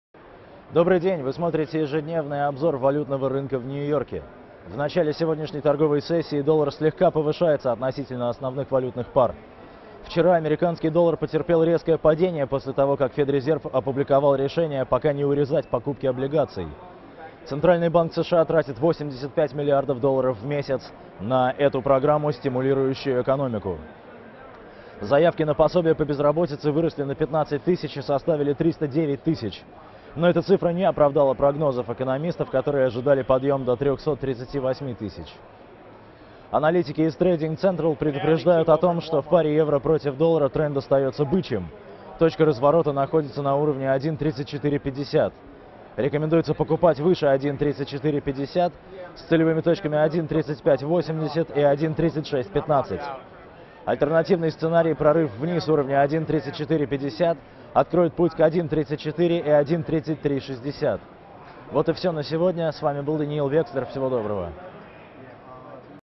Доллар повышается, после вчерашнего падения
Федрезерв не станет понижать скупки облигаций; рост заявок на пособие по безработице не оправдал ожиданий